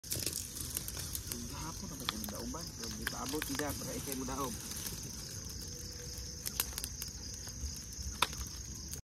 ProjectFarm - Daub sa mga dahong laya
Akung gikumusta ang akung 13th month nga imbis pang iphone14 sa uban, akung gi palit ug 10 ka baktin unya patukod ug payag.